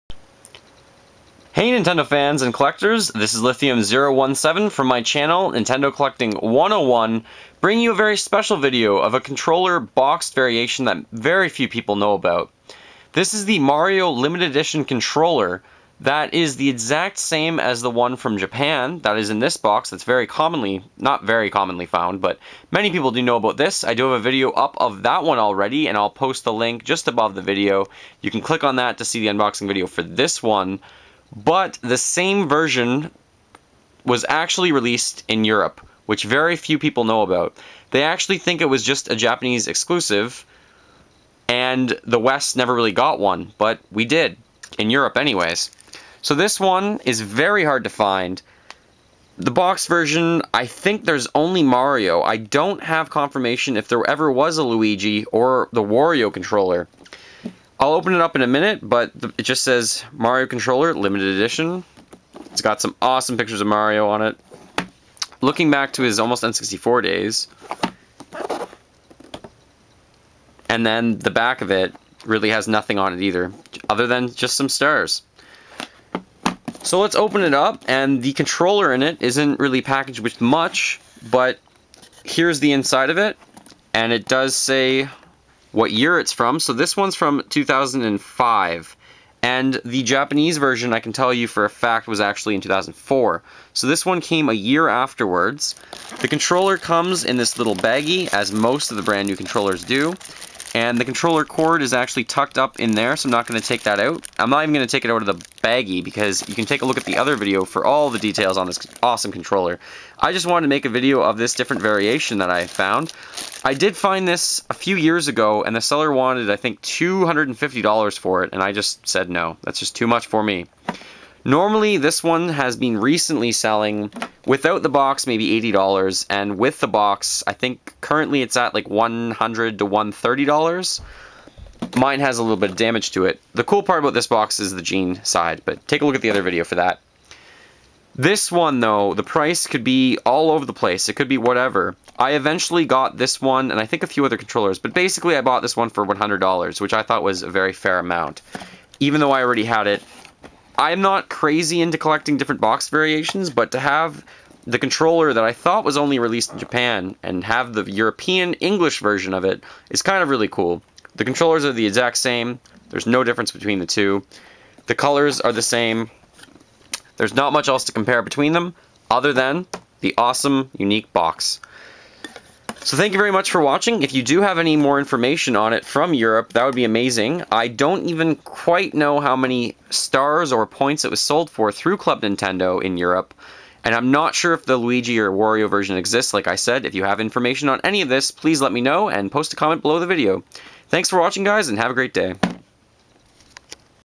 Mario Gamecube Controller from Club Nintendo Europe
This very rare controller was more commonly known to be released in Japan. I display both boxed versions in this video